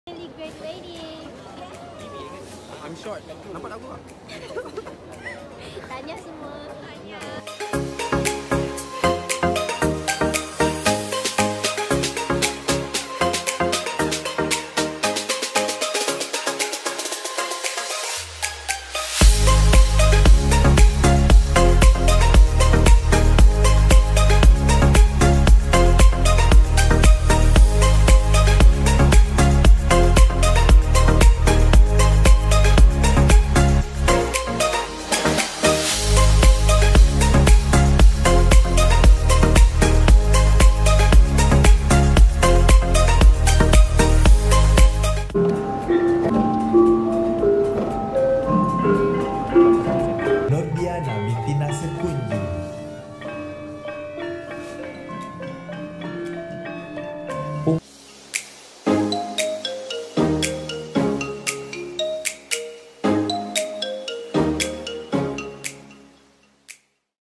Convocation Day
Alhamdulillah🎓 Officially graduated from Universiti Selangor in Bachelor of Environmental Science (Biodiversity & Conservation) 👩🏻‍🎓(Hons) @ Dewan Canselor, Universiti Selangor Kampus Bestari Jaya [30 November 2019]